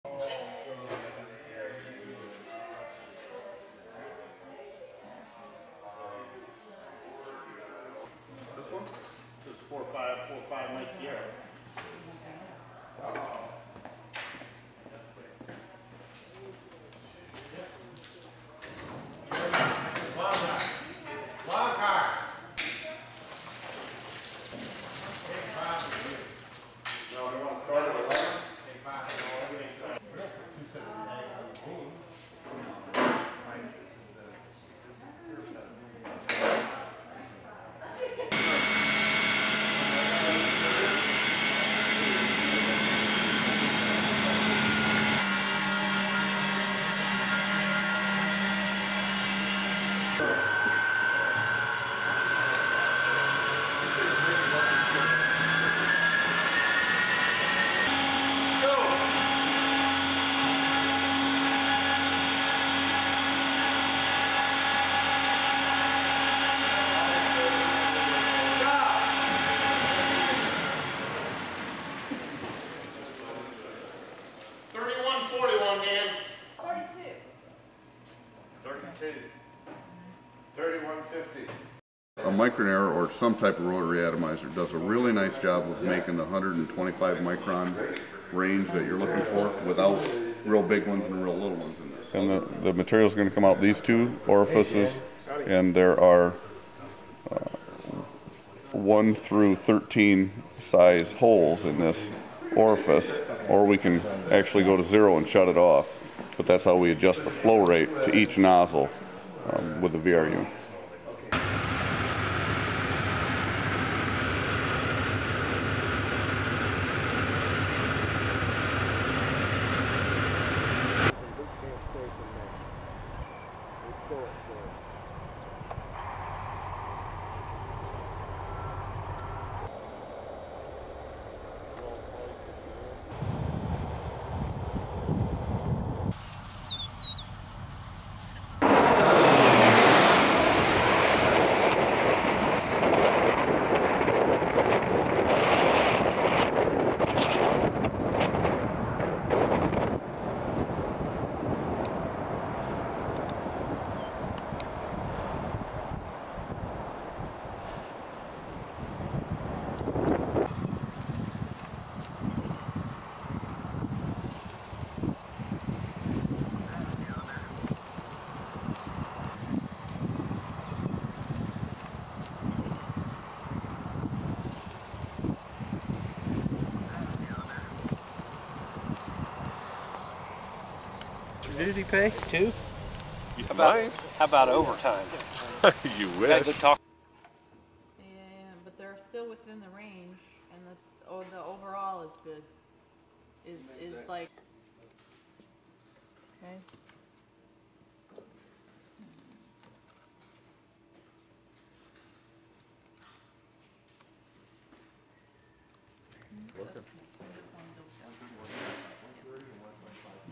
Aicraft Calibration and Characterization
Calibration of aircraft boom with Micronair AU500 atomizers and characterization of spray. Technique is used by most fix-wing aircraft conducting gypsy moth control in the eastern US.